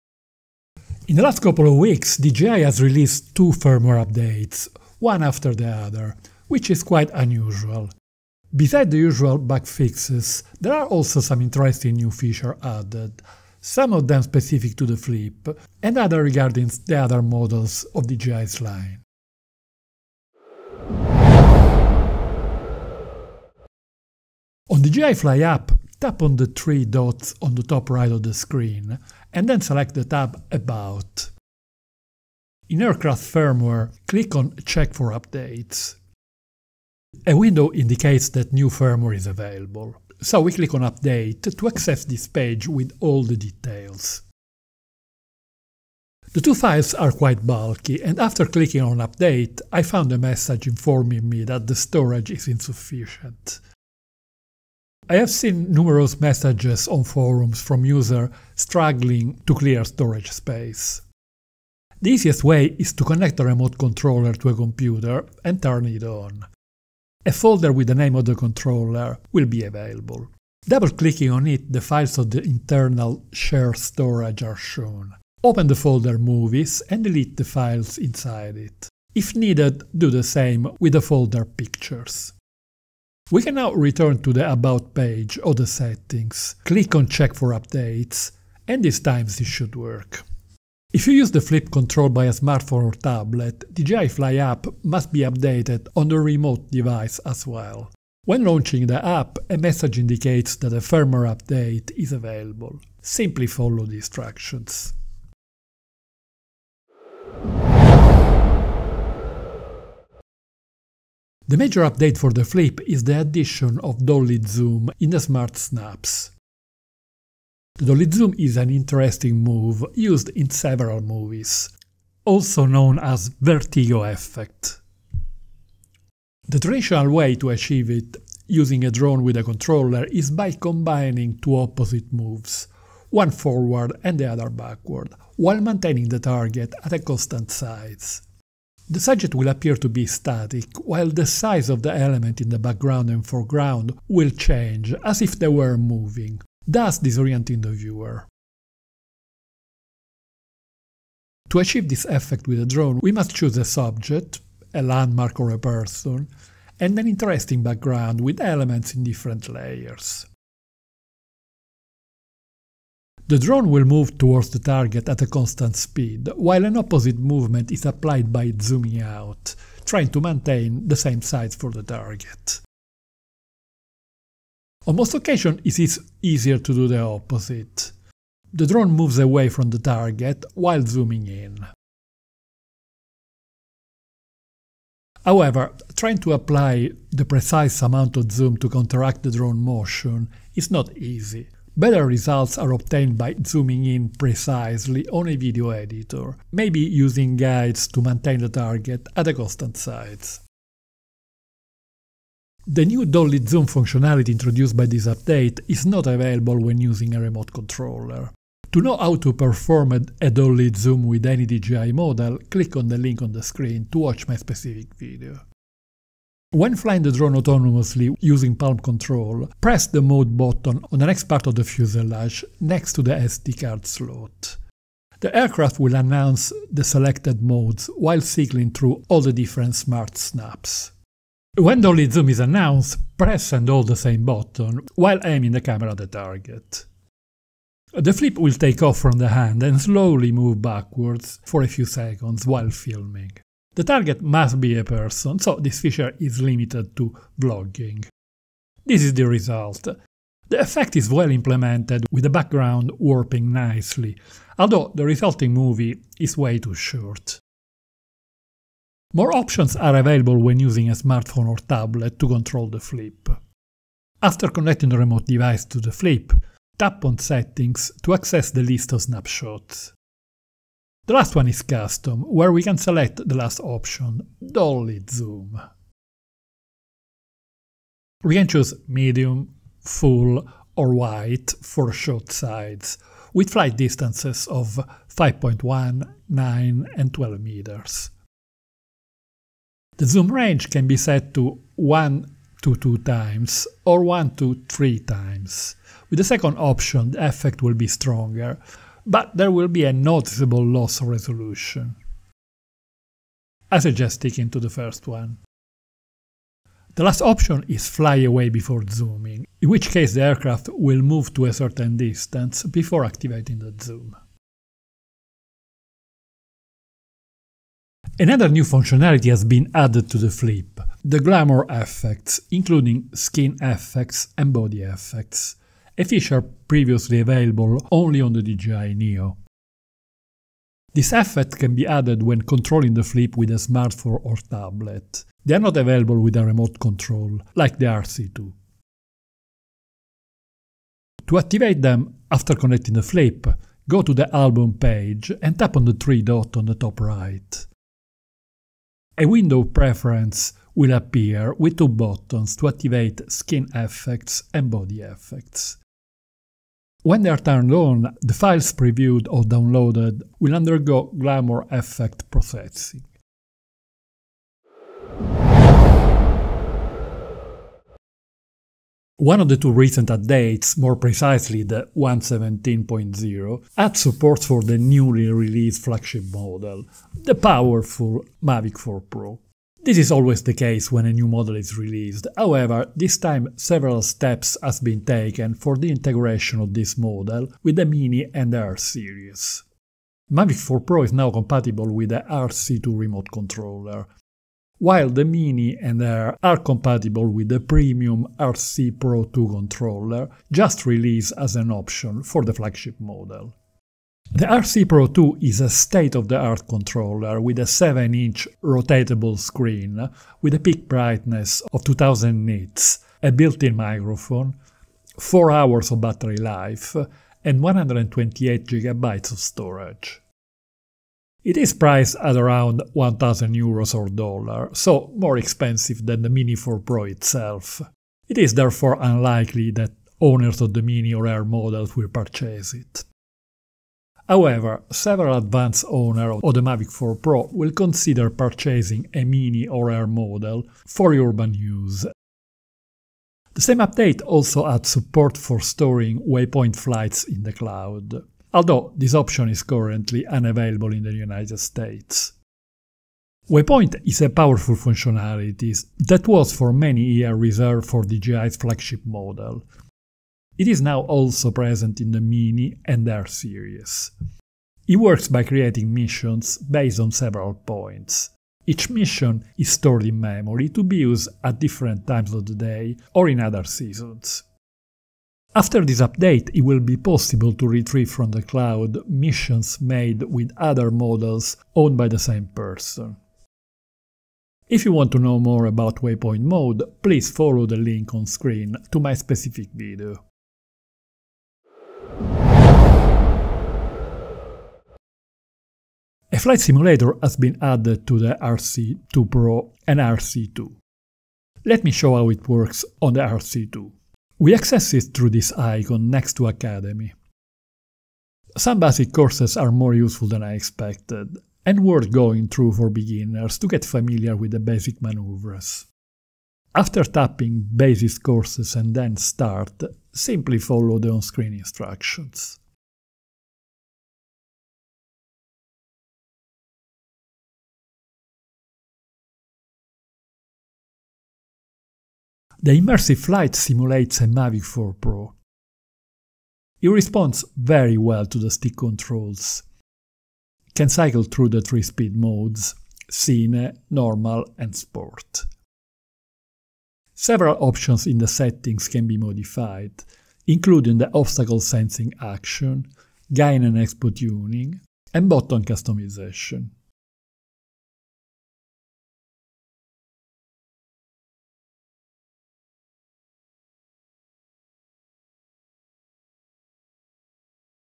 DJI Fly App Firmware Updates 1.17.0 and 1.17.1 including Dolly Zoom and Flight Simulator
In the last couple of weeks, DJI has released two firmware updates for the Fly app, 1.17.0 and 1.17.1. Besides the usual bug fixes, there are also some interesting new features added, like Dolly Zoom functionality and a flight simulator

DJI Flip
DJI Mini 4 Pro
DJI Mini 3
DJI Mini 4k
DJI Neo

0:00 Intro
0:21 How to update
1:47 DJI Flip
6:04 Other models
8:12 Flight simulator